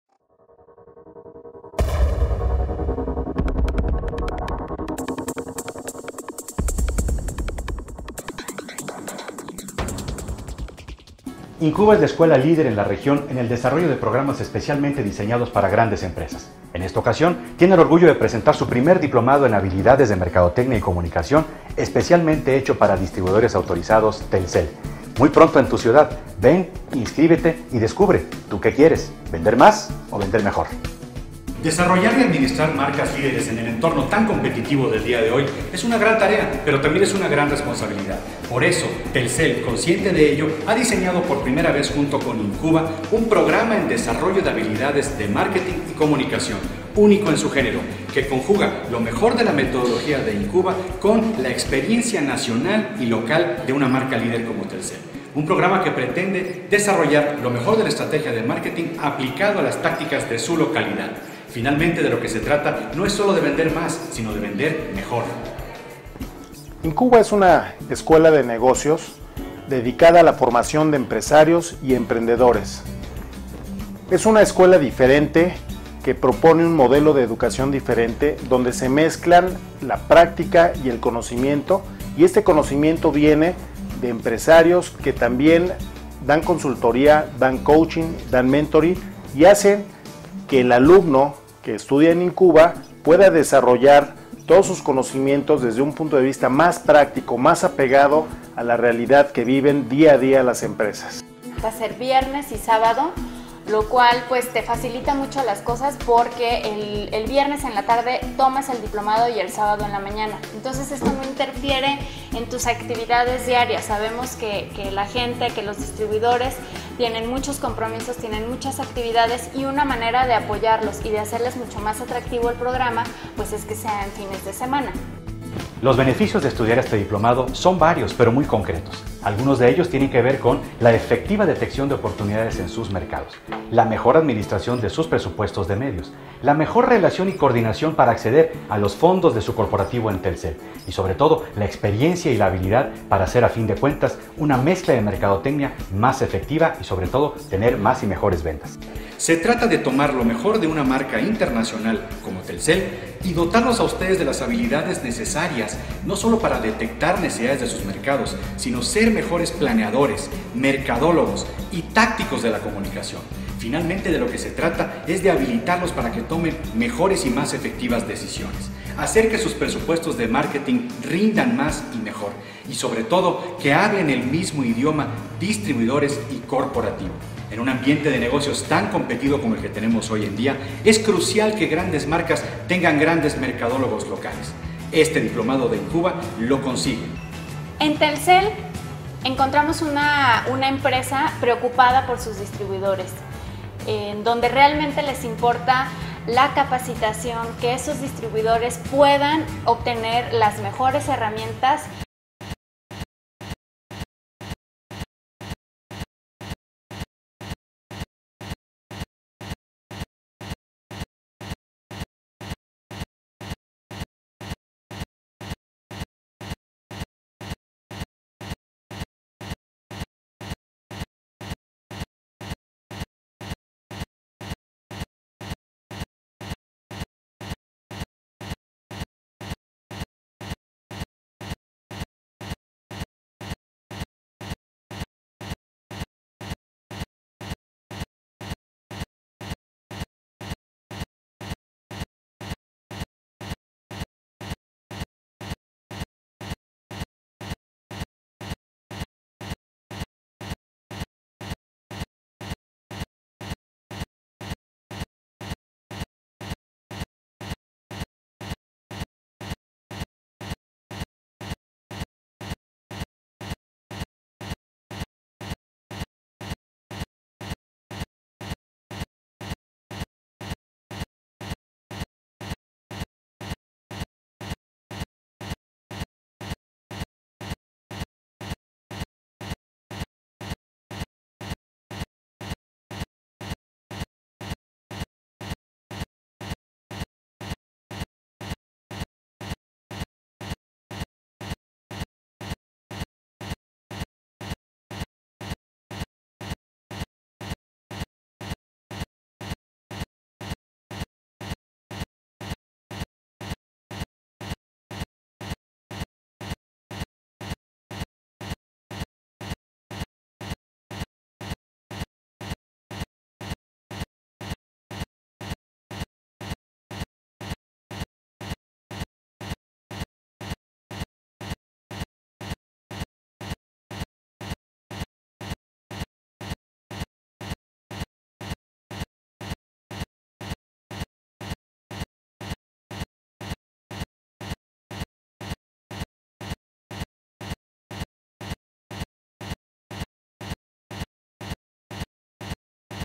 DIPLOMADO DE MARKETING Y COMUNICACION PARA TELCEL
Video informativo y testimonial del Diplomado en Marketing y Comunicación impartido a Telcel en InQba